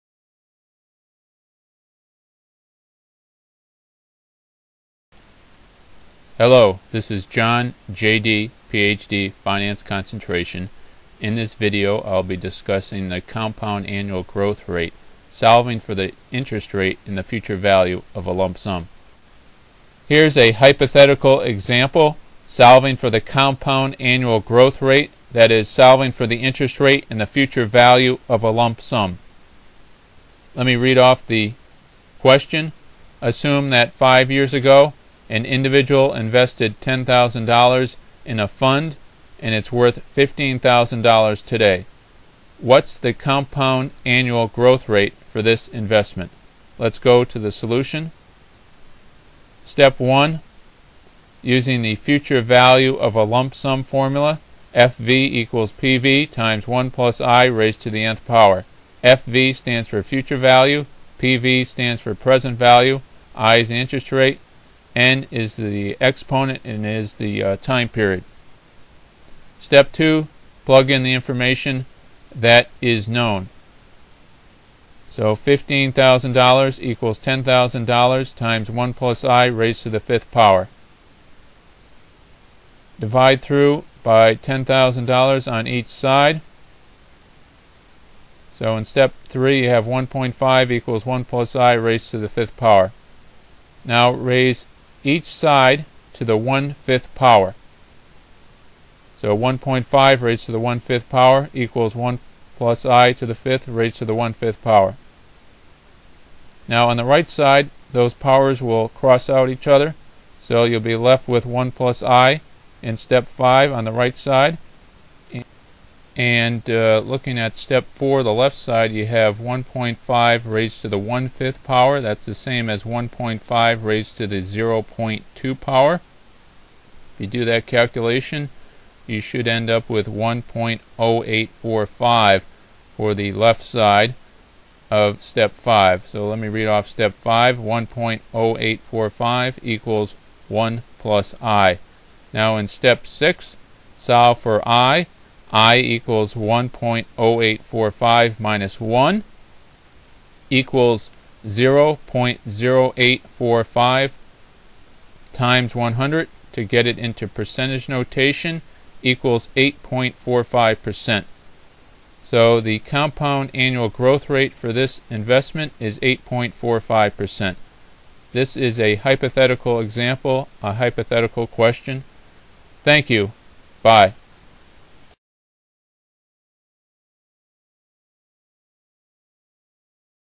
Compound Annual Growth Rate (Solving for "i" in the FV of a Lump Sum) by John, J.D., Ph.D.
Compound Annual Growth Rate: Solving for the Interest Rate in the Future Value of a Lump Sum
by John, J.D., Ph.D.